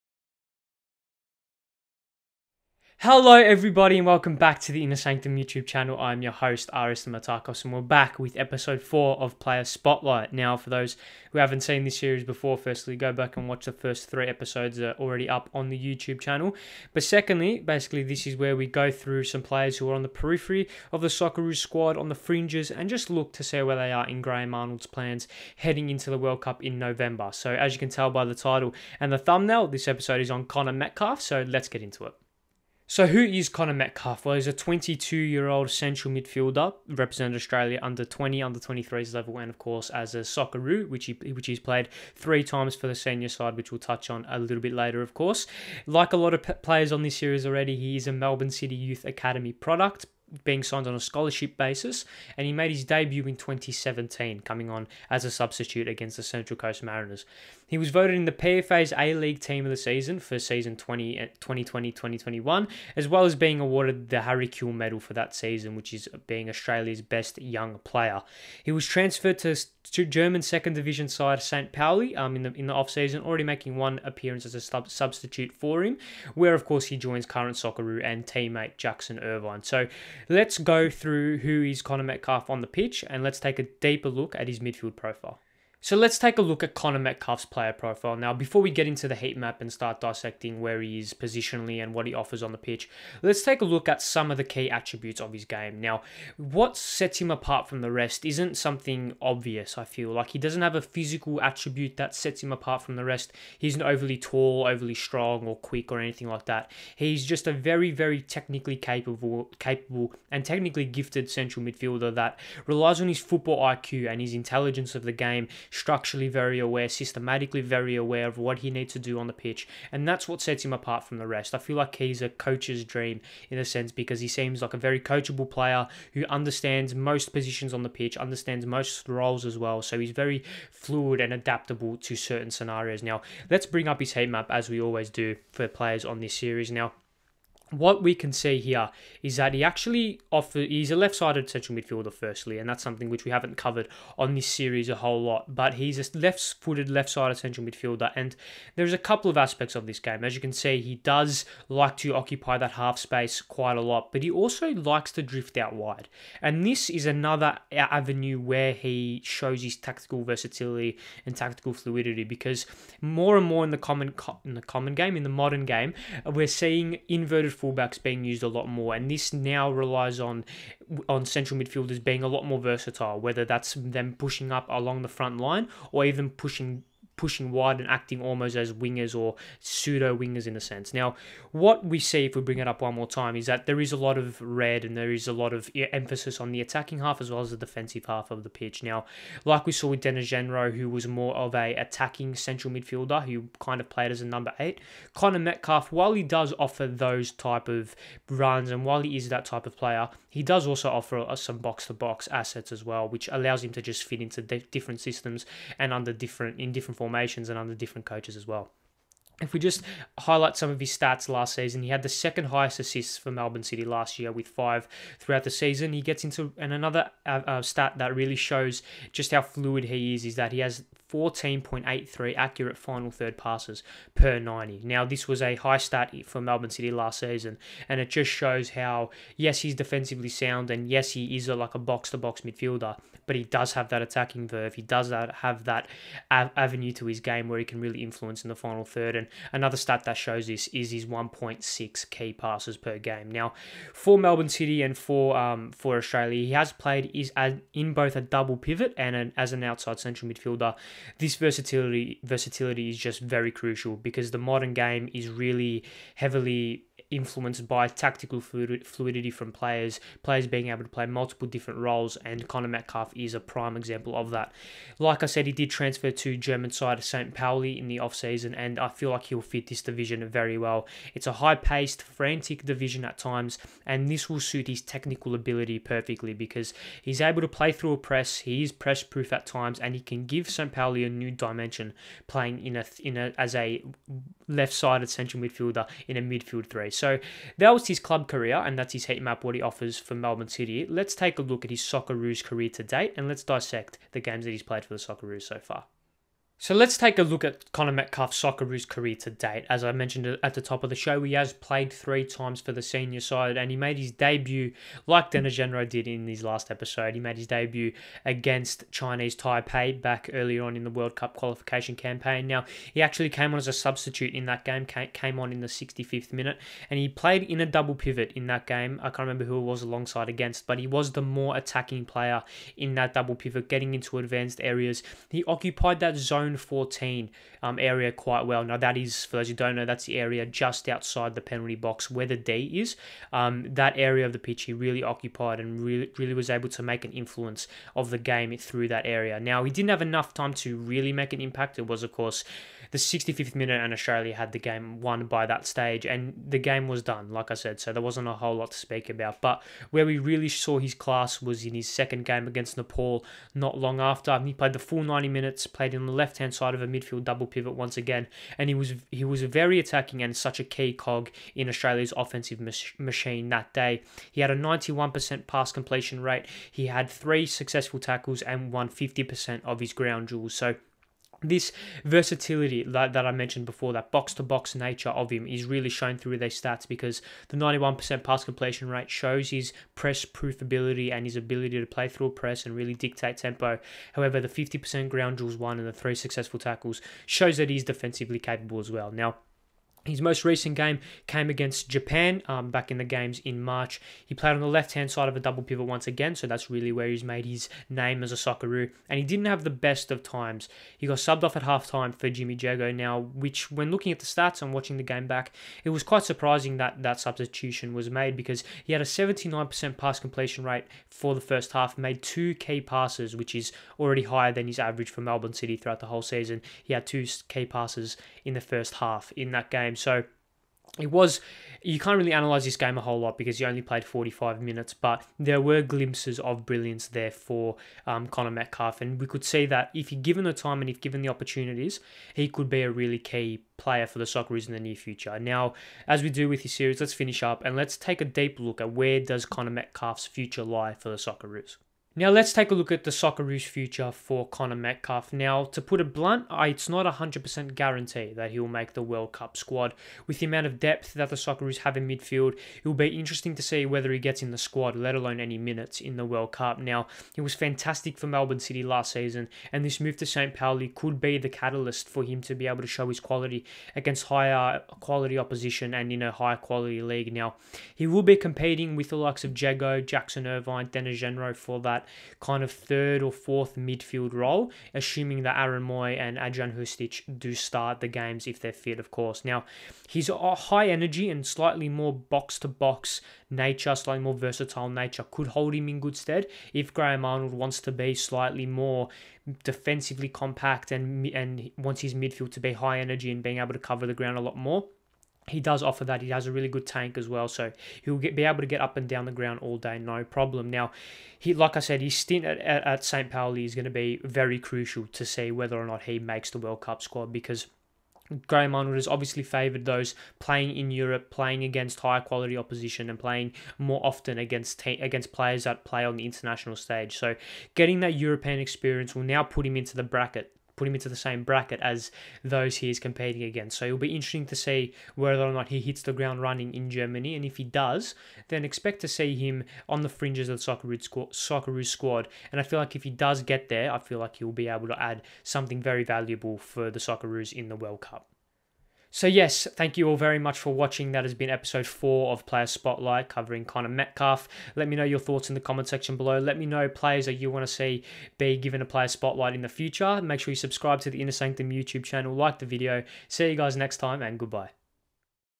PLAYER SPOTLIGHT | EPISODE 4: CONNOR METCALFE
In this video, Ari Stamatakos does a tacitical deep dive into young Socceroos player Connor Metcalfe and his prospects heading into the World Cup in November